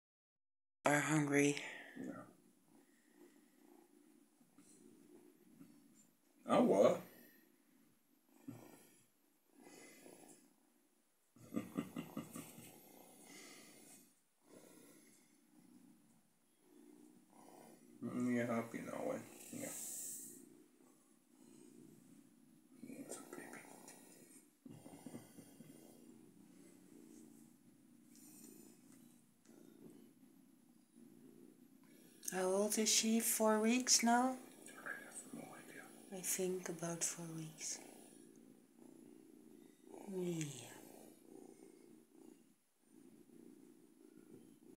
On Sept 21we found this poor baby kitten on the street. She was very upset in panic and screaming
"Please feed me! I am starving, all alone and afraid".We now have 11 cats who need their bellies fed every day and, of course, all need their vaccinations, deworming and sterilisation. If you can (and want to) help, we hope  you can donate a little to ease our burden... Please.